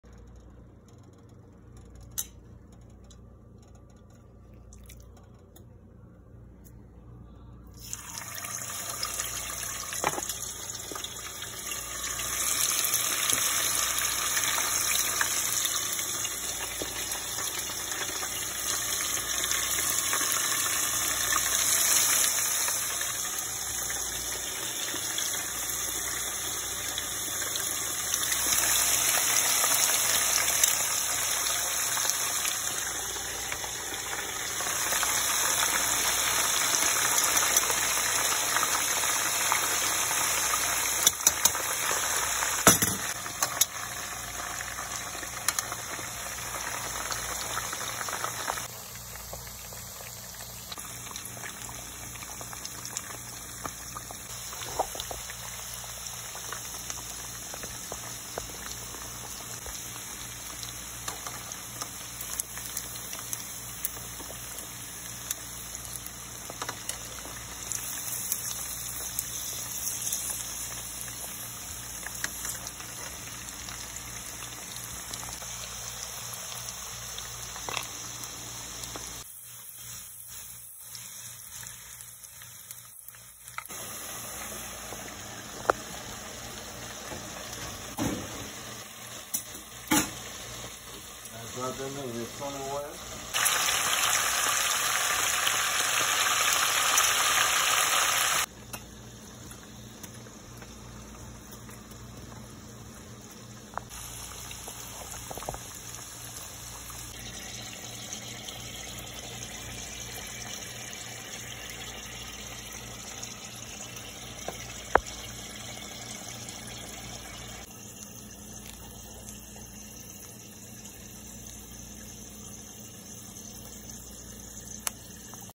Naked fried pig tails 🚫No flour 🚫No boiling
Wash and seasoned pig tails marinated for a hour or two and dropped straight in the grease. 🚫No boiling,🚫no flour. With a side of left over risotto.